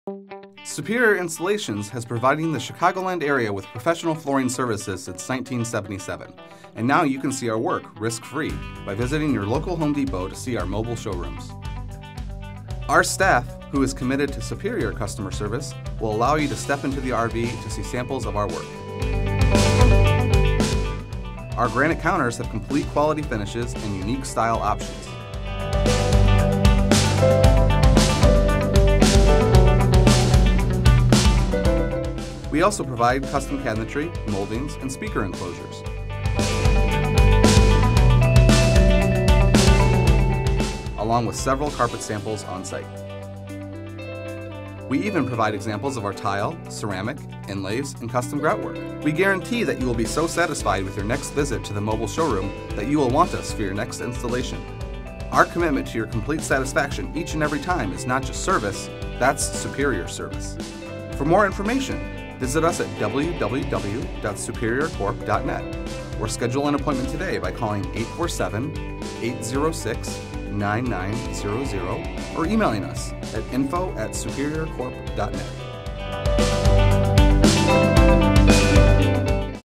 Superior Corporation - Mobile Showrooms
An overview of our mobile showrooms, available soon at your local Home Depot.